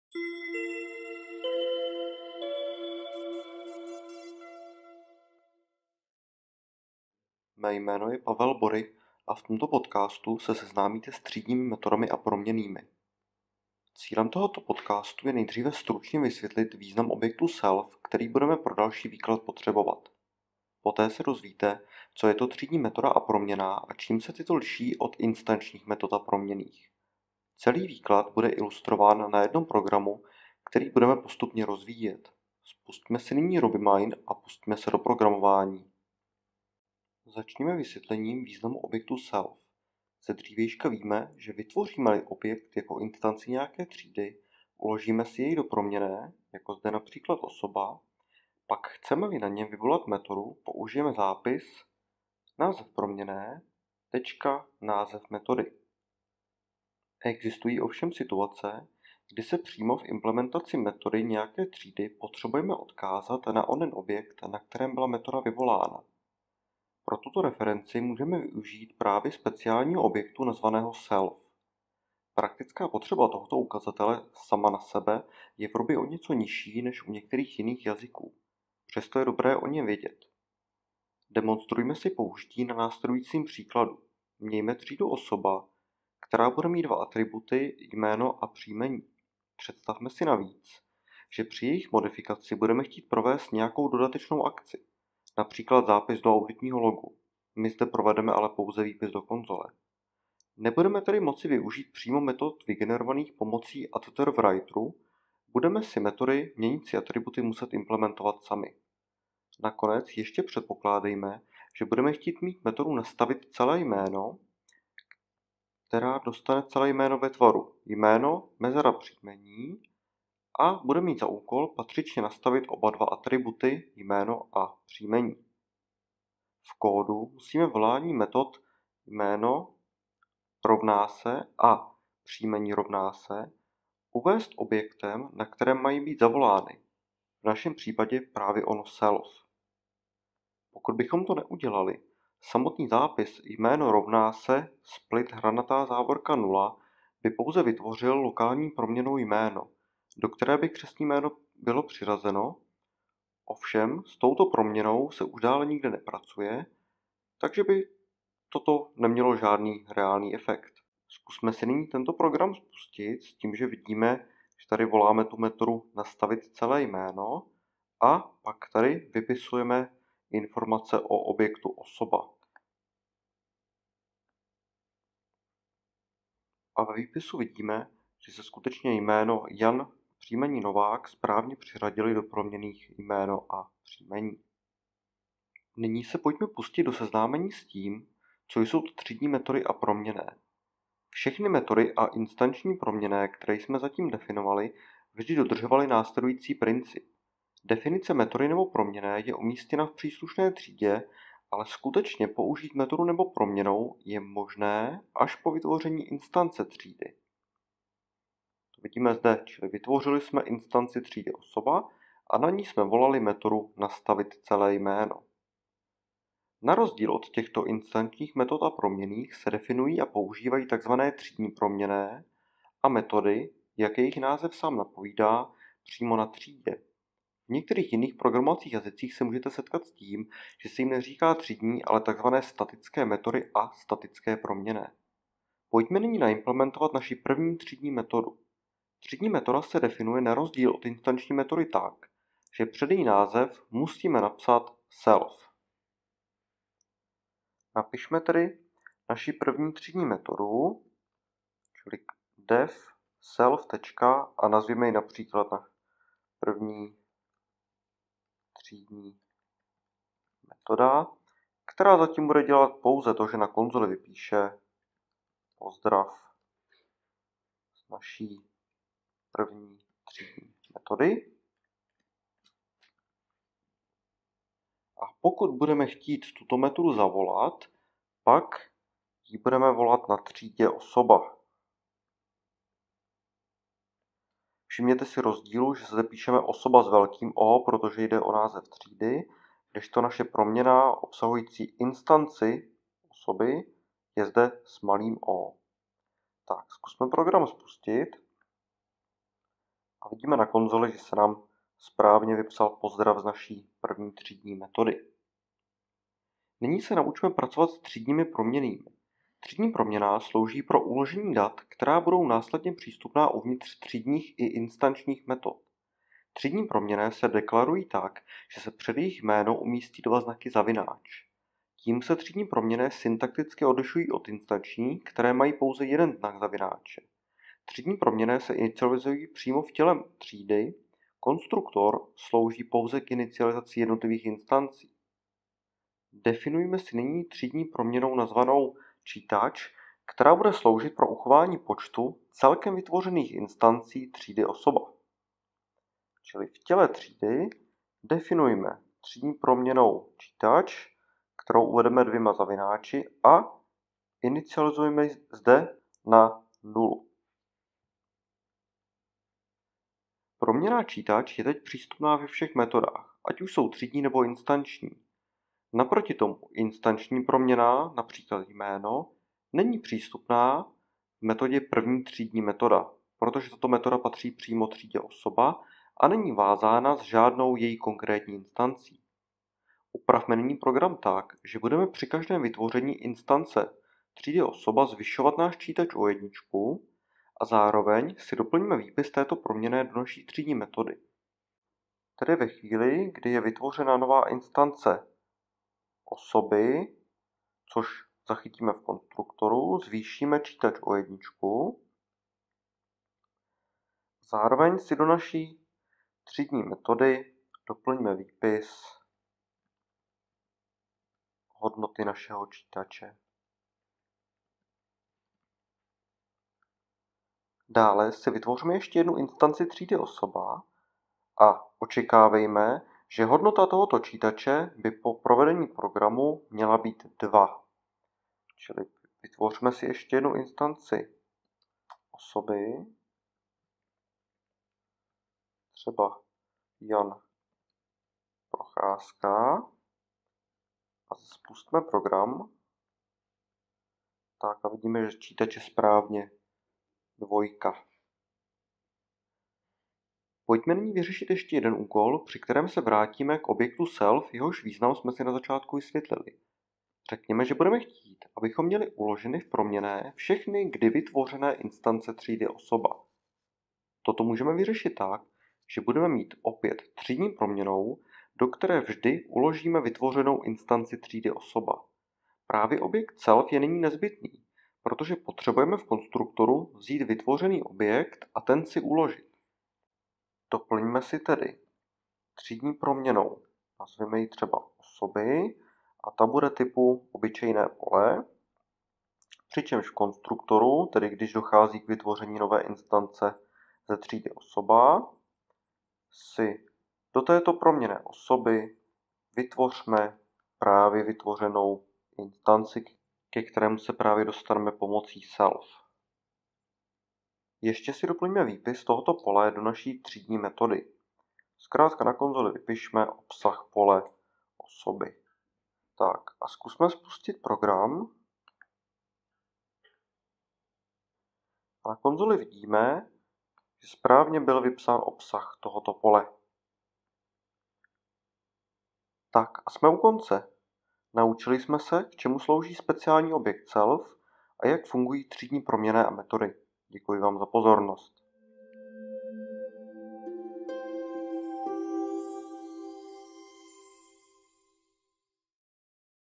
Třídní metody a proměnné - Základy objektově orientovaného programování
Význam objektu self, třídní metody a třídní proměnné. Výklad je ilustruván na postupně programovaném příkladu.